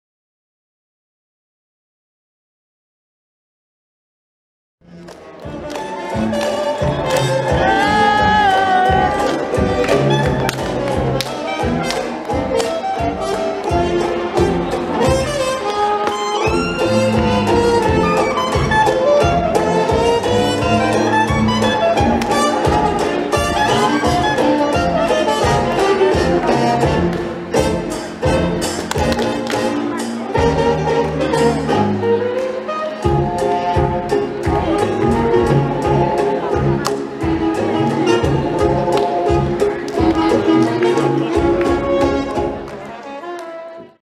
The Cat's Pyjamas at the Hooley in the Hall, Cork
Lindy Hop and Solo Jazz performance by The Cat's Pyjamas at the Hooley in the Hall June 2016.This was a  charity event in aid of Pieta House and the Mercy Hospital.